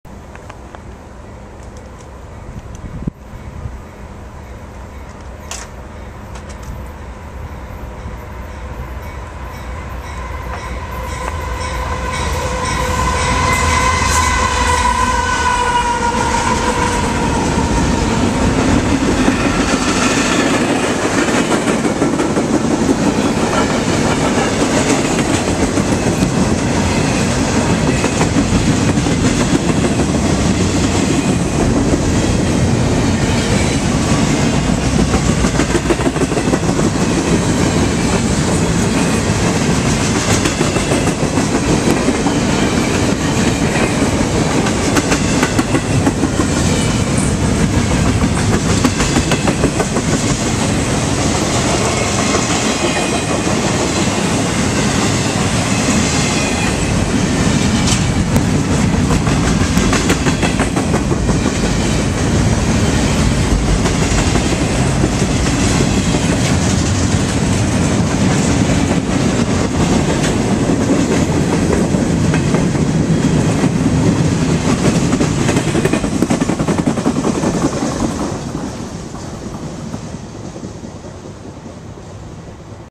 CSX L128 BARRELS THRU YARDLEY PA (All Well Cars)
6/26/13 I decided to stop in at Yardley Station today. Got there at 12:45 pm and at 12:50pm a horn can be heard from an approaching Eastbound. It was L128. L128 was all Empty Well Cars. The power on L128 was CSX #668 (CW60AC) leading and #876 (ES44AH). Very cool move.